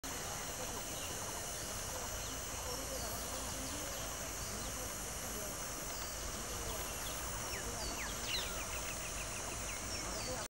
Neurothemis Tullia (Male) Dragonfly
The Pied Paddy Skimmer (Neurothemis tullia) is a species of dragonfly found in south and south-east Asia. It appears in Bangladesh, China, Hong Kong, India, Malaysia (Peninsular Malaysia), Myanmar, Nepal, Sri Lanka, Taiwan, Thailand, and Viet Nam. The species is fairly common, with a black body and black and white wings with transparent tips.